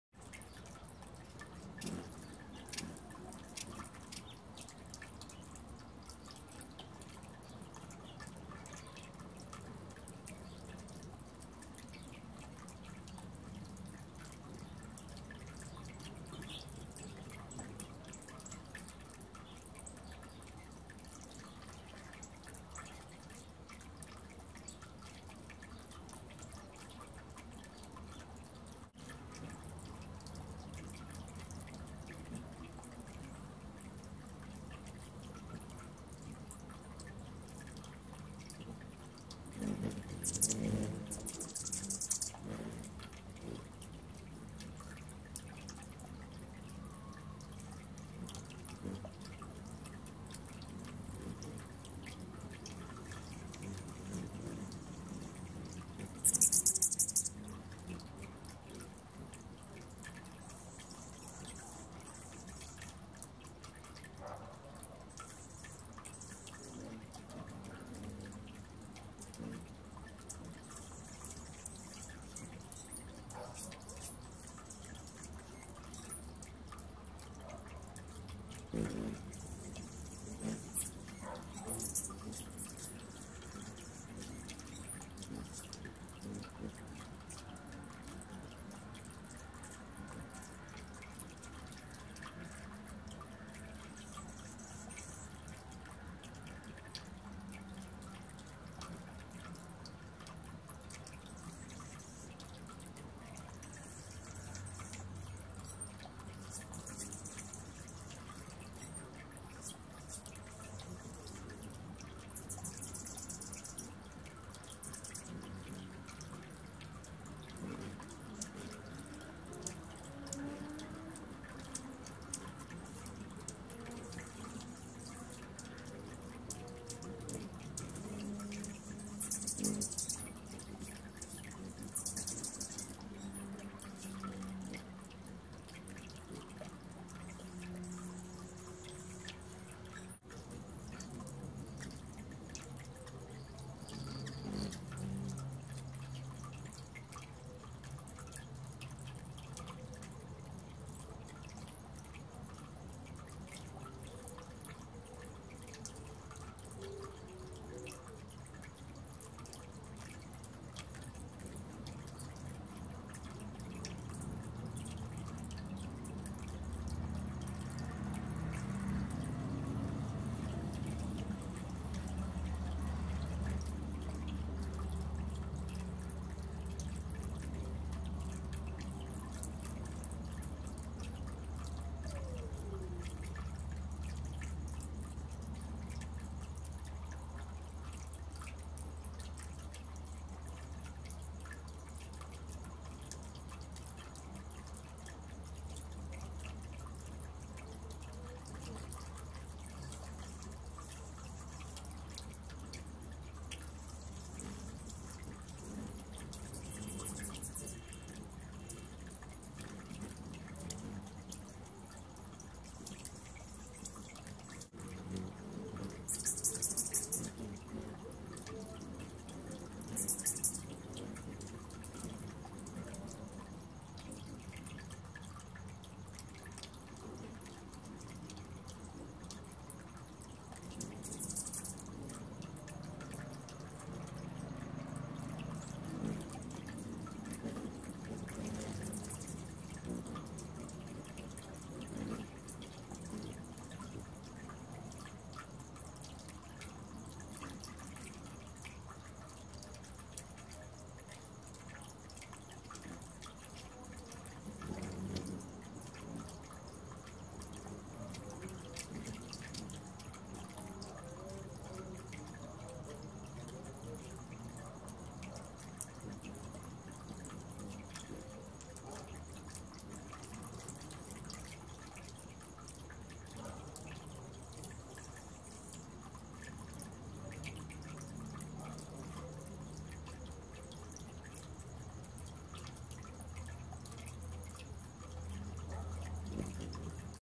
Microteaching Hummingbird Identification
Try to identify as many hummingbird species that visit the feeder as you can.  Write them down on your bird checklist!